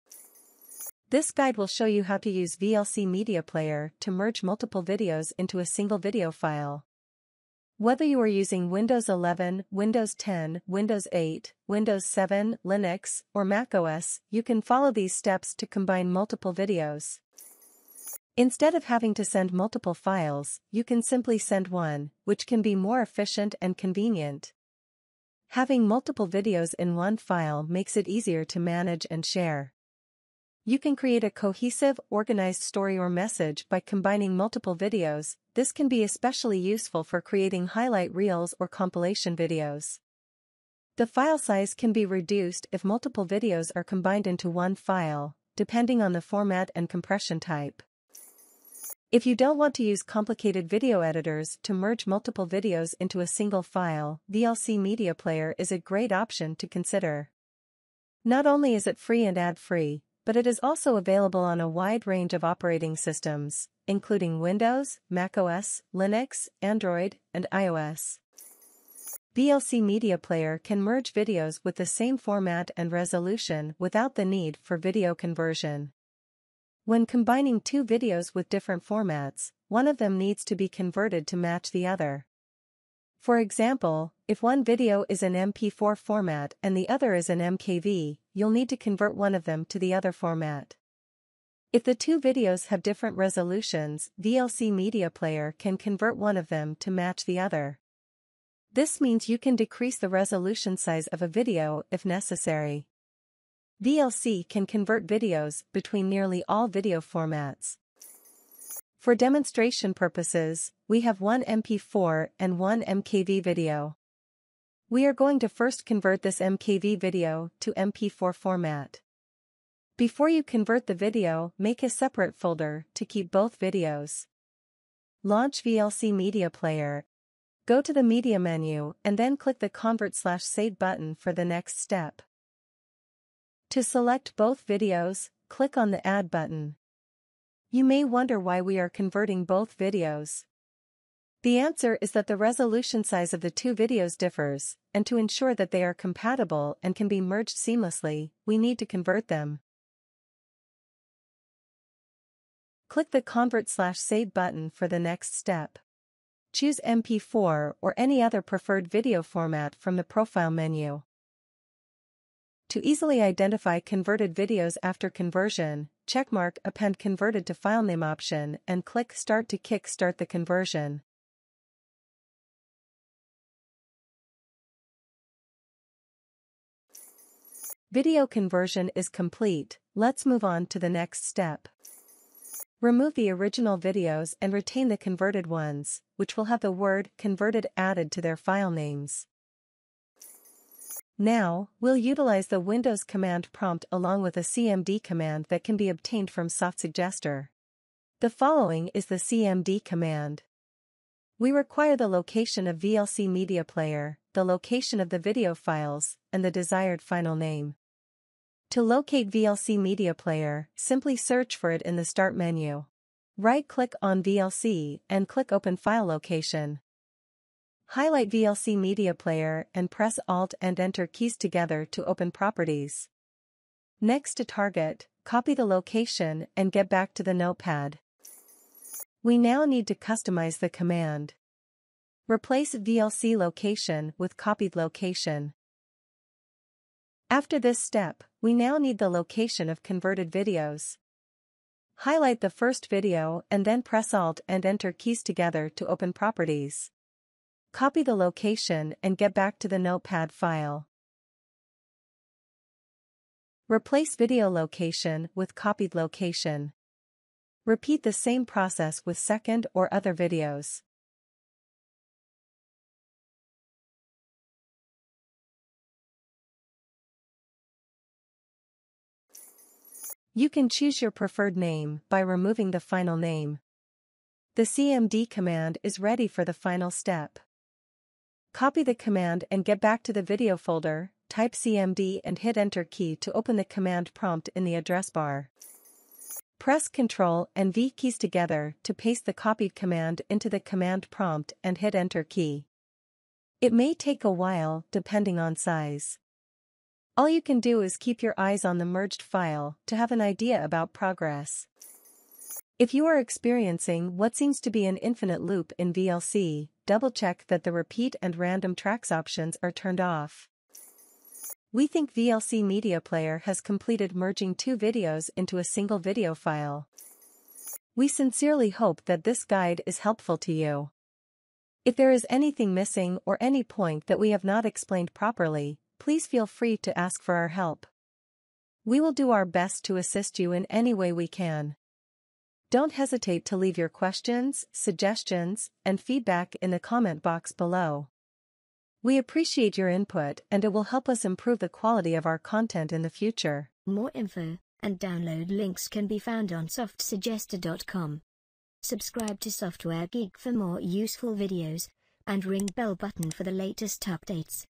How To Use VLC To Merge Video Files Into One Single File?
In this video, we'll show you how to use VLC Media Player to merge multiple videos into a single video file quickly and easily. Whether you're using Windows 11, Windows 10, Windows 8, Windows 7, Linux, or macOS, you can follow these steps to combine multiple videos.

If you don't want to use complicated video editors to merge multiple videos into a single file, VLC Media Player is an excellent option to consider. Not only is it free and ad-free, but it's also available on a wide range of operating systems, including Windows, macOS, Linux, Android, and iOS.

While we're not sure about Android or iOS, you can use VLC Media Player to merge your videos on Windows, macOS, and Linux. What's more, VLC Media Player is incredibly user-friendly, making it an ideal choice for those who find dedicated video editors challenging.

You don't need any special technical knowledge or experience to use VLC Media Player, as its intuitive interface guides you through the process step by step. VLC Media Player can merge videos with the same format and resolution without the need for video conversion.

However, when combining two videos with different formats, one of them needs to be converted to match the other. For example, if one video is in MP4 format and the other is in MKV, you'll need to convert one of them to the other format. If the two videos have different resolutions, VLC Media Player can convert one of them to match the other. This means you can decrease the resolution size of a video if necessary. VLC can convert videos between nearly all video formats.

So, if you want to merge multiple videos into a single file quickly and easily, without the need for complicated video editors or technical knowledge, follow the steps in this tutorial and use VLC Media Player.